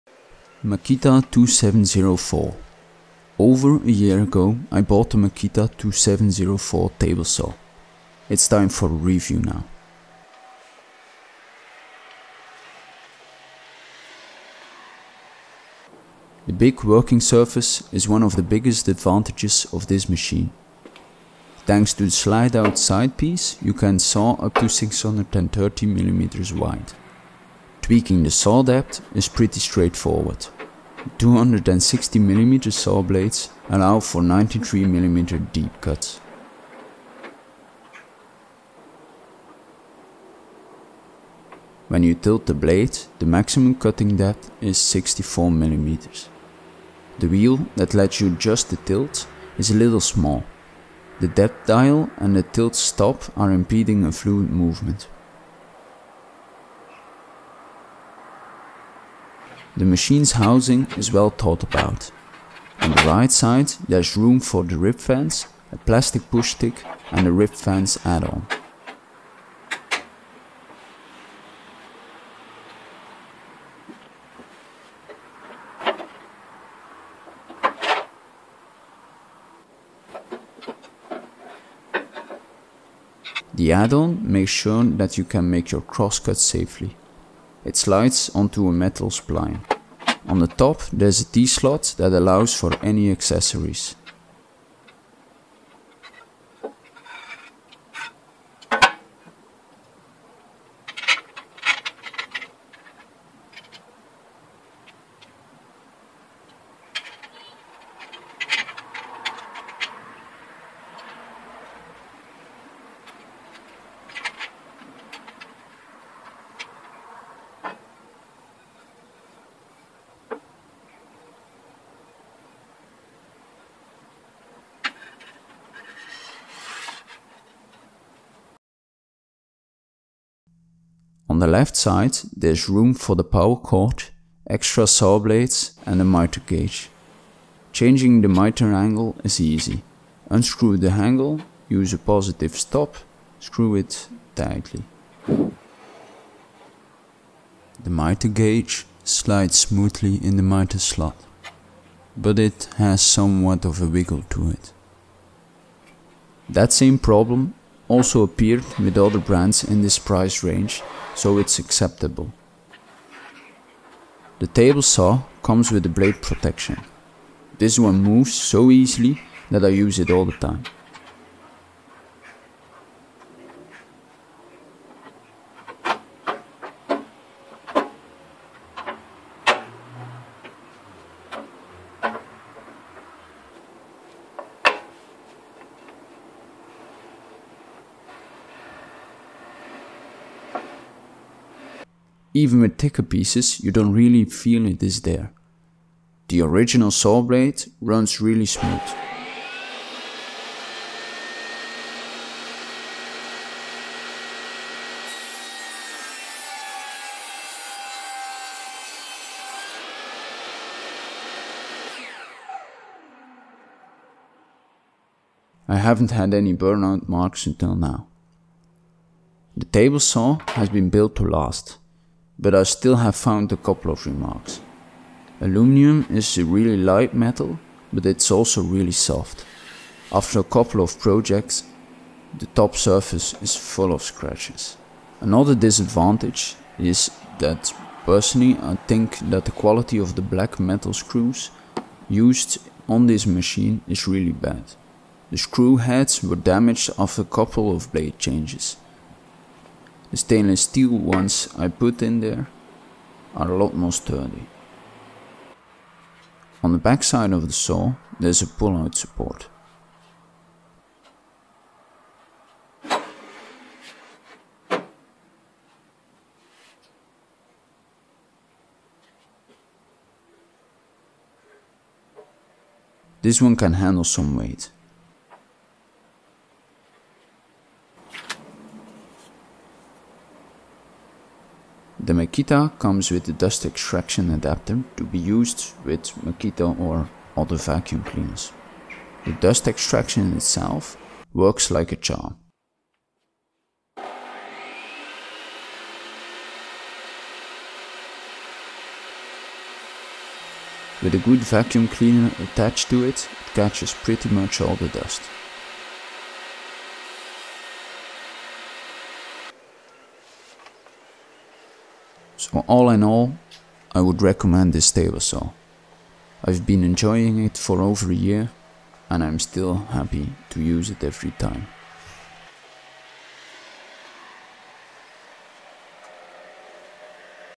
Makita 2704 table saw
I've been using the Makita 2704 table saw for quite some time now. I've done a review in dutch in the past. A lot of people requested me to do an English review too.